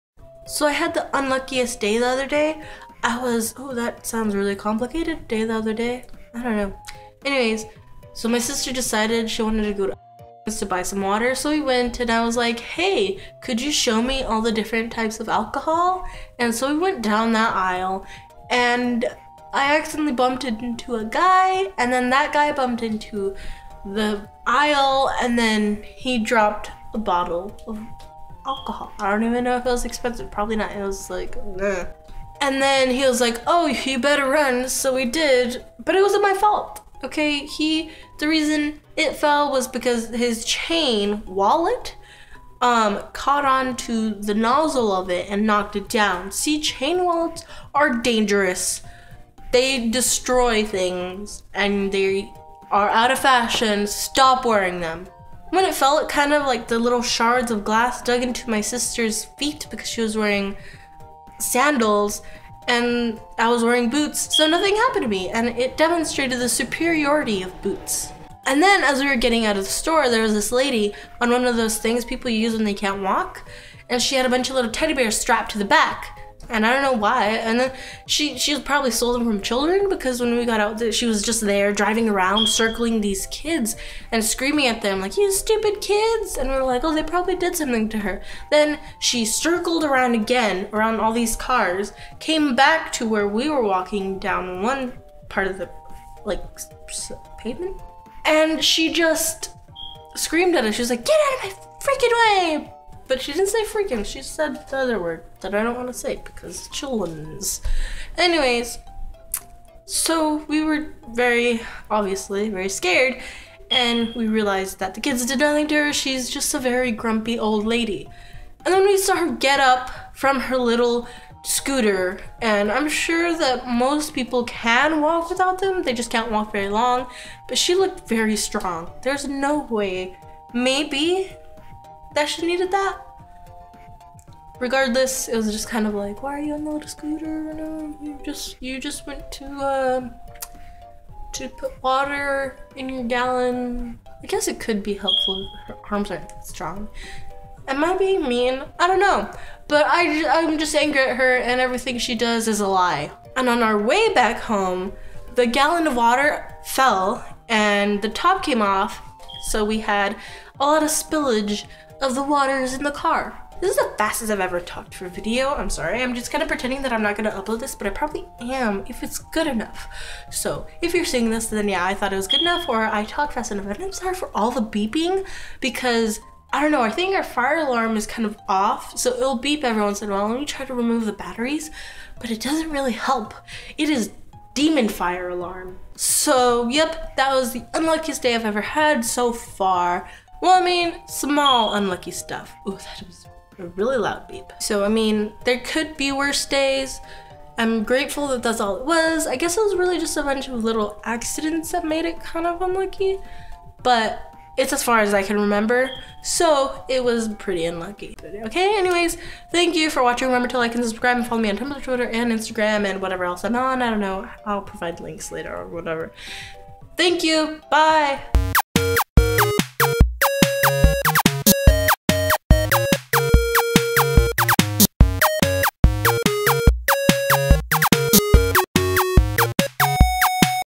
Unluckiest Day Ever
This was just a quick vlog thingy I did when I did the intro for my last video, I wasn't going to upload it, but it's okay, so I'm doing it anyways!